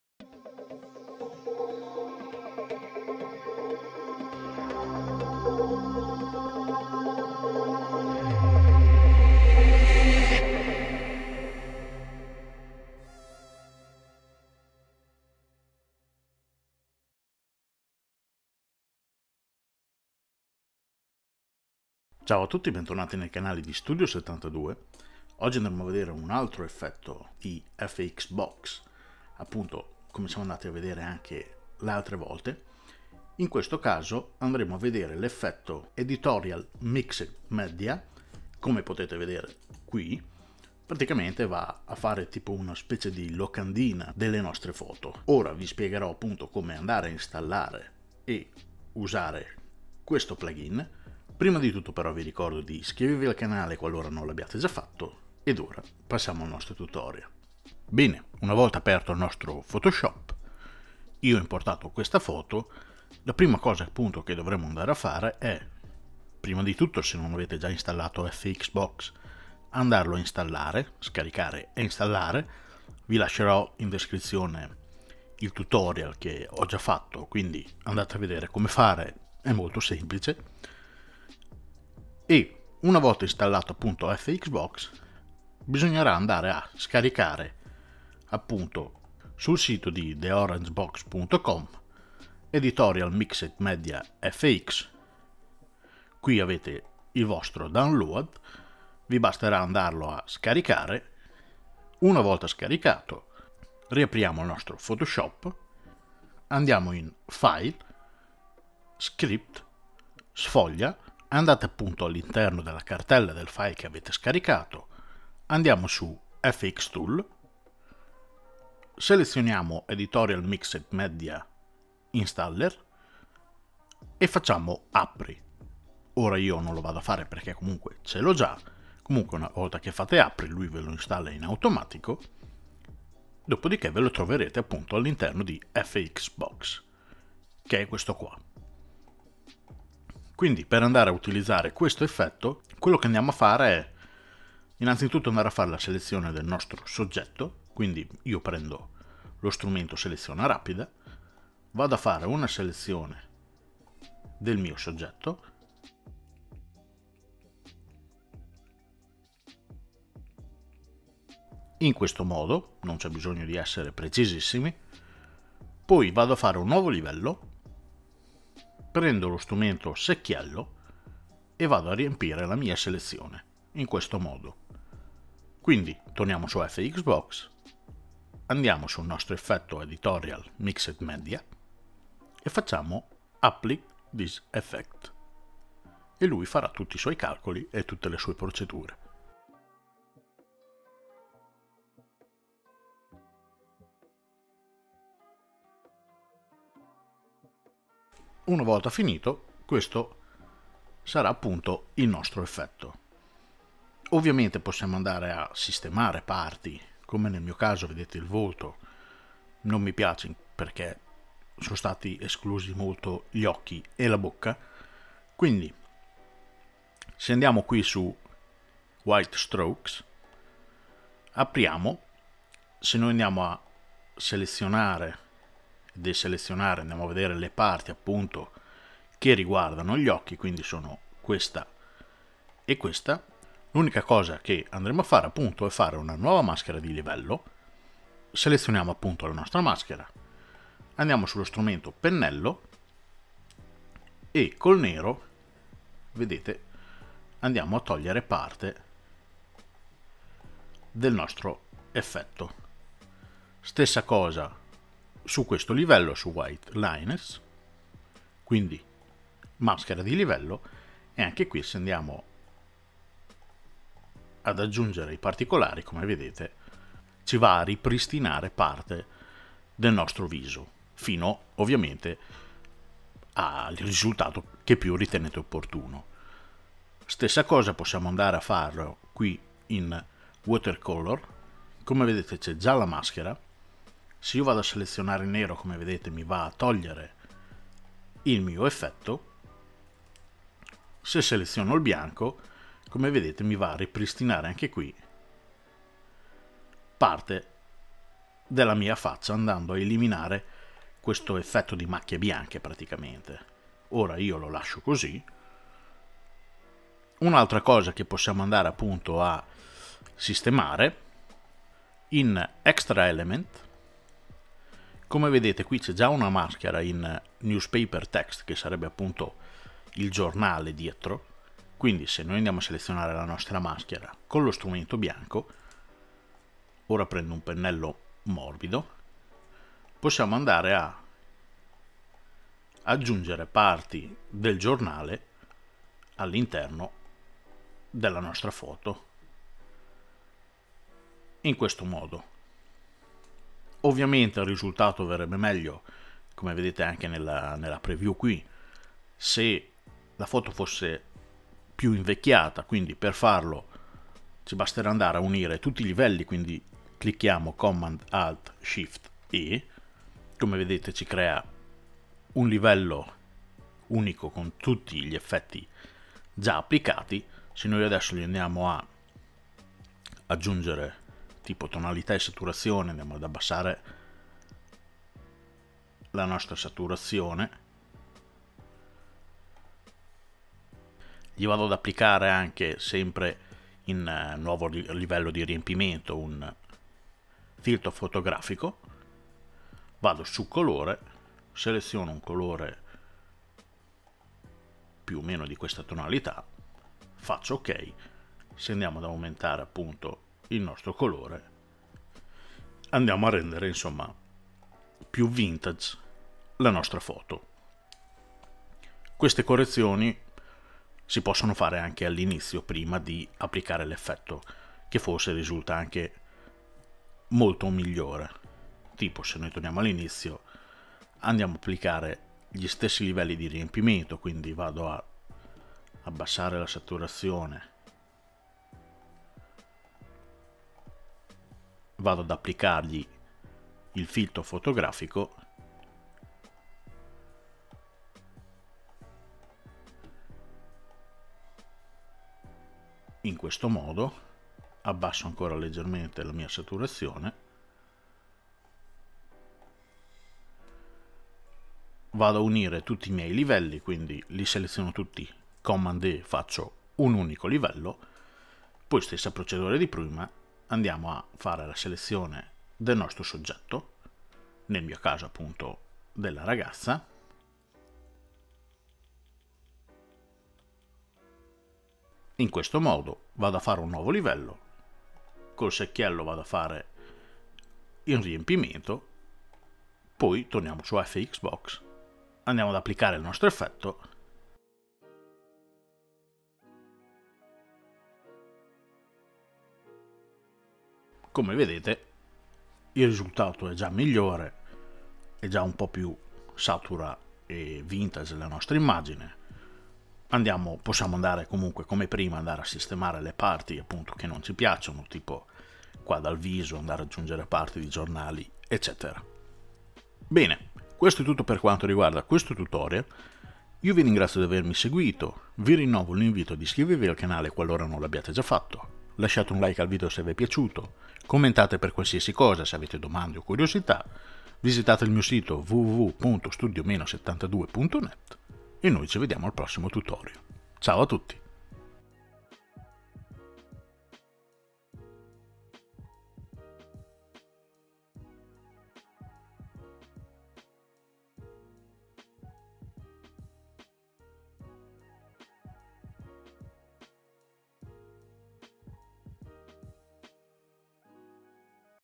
Adobe Photoshop CC - Tutorial FX Box & Editorial Mixed Media FX download [ITA], Studio72
Vuoi realizzare un effetto realistico di locandina su una tua foto? Editorial Mixed Media FX è quello che ti serve. Ti spiegherò in modo facile come scaricare, installare e usare FX box & Editorial Mixed Media FX per realizzare splendide foto con effetto locandina su Adobe Photoshop CC.

Powered by - Studio72